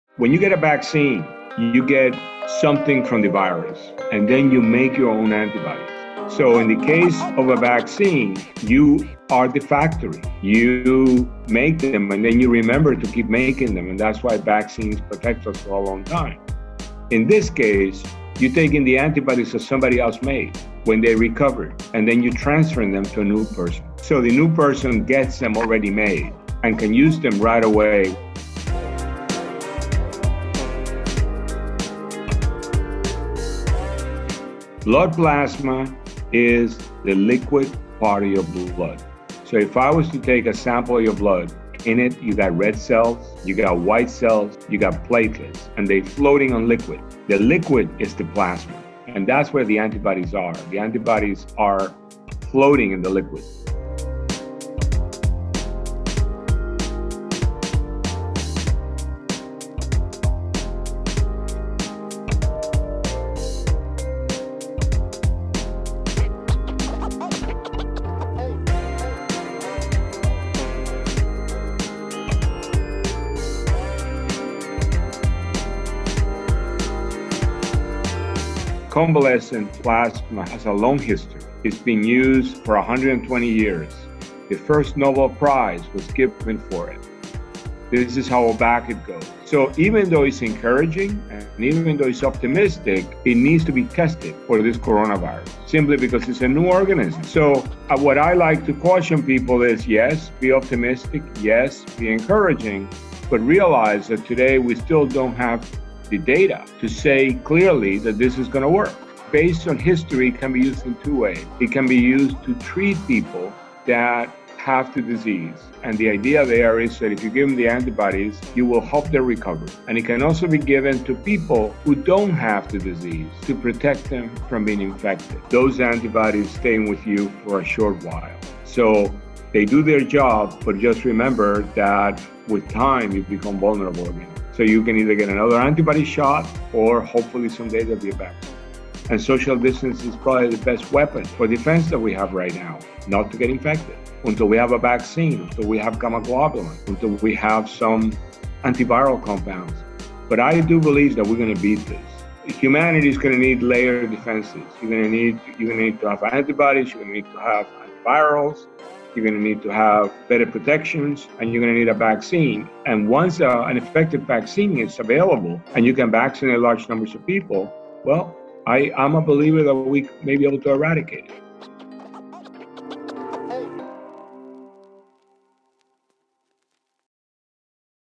What is Convalescent Blood Plasma Therapy? | Bloomberg Philanthropies
Johns Hopkins Bloomberg School of Public Health continues to be at the forefront of the battle against COVID19. Today, they received $35M in funding from the U.S. Department of Defense (DoD) for two nationwide clinical trials led by Dr. Casadevall to test the effectiveness of convalescent plasma as an outpatient treatment.

“This is a story of great synergy between researchers and institutions to carry out important studies that will inform our nation and the world on how effective plasma can be to prevent COVID-19 and to treat early disease,” said Dr. Arturo Casadevall, Johns Hopkins Bloomberg School of Public Health Professor and Chair of the Department of Molecular Microbiology and Immunology.

Learn more about the trials and why this is an important step in fighting this pandemic